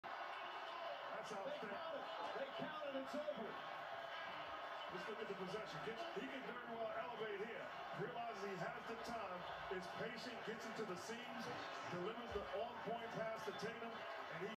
Jayson Tatum buzzer beater 2022 play offs
Boston won the game with jayson tatums buzzer beater